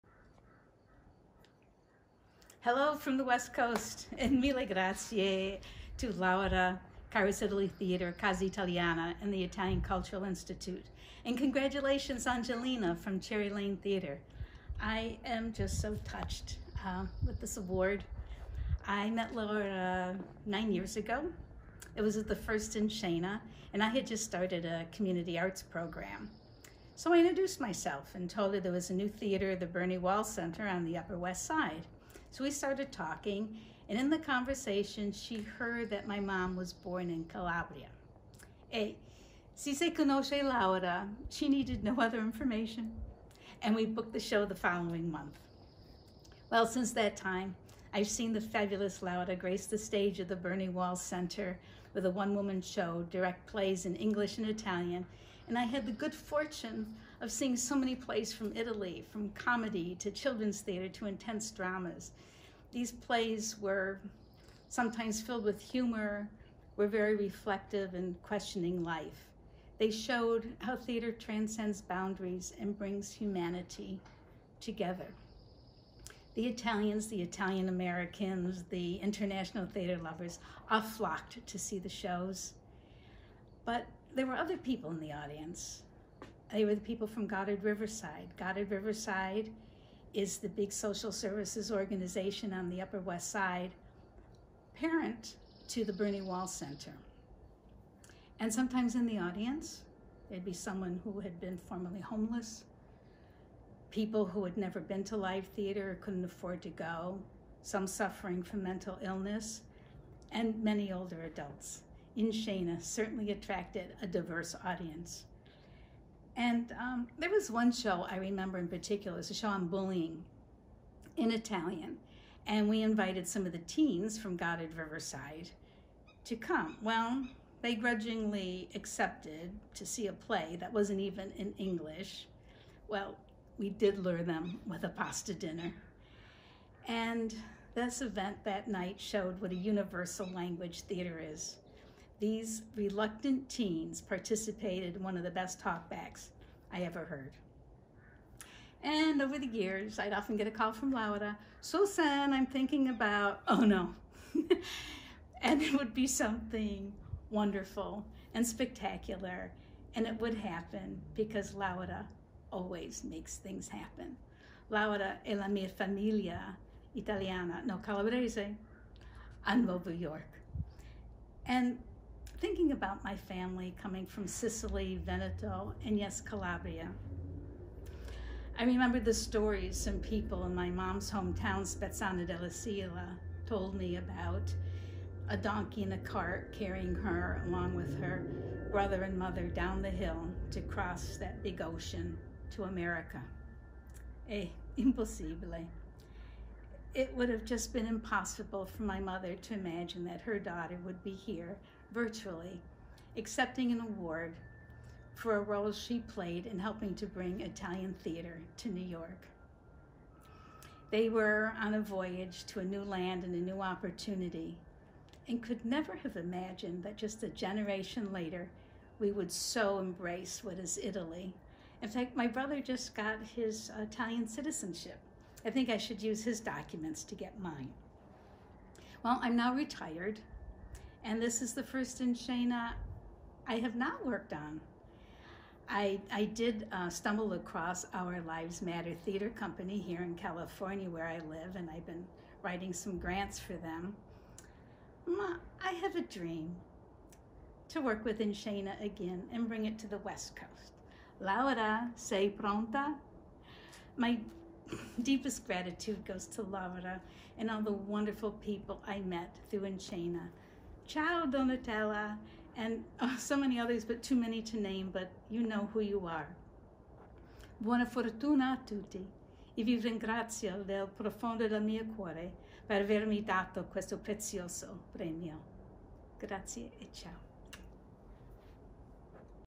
Susan Macaluso
Video acceptance by Susan Macaluso, In Scena Award 2022.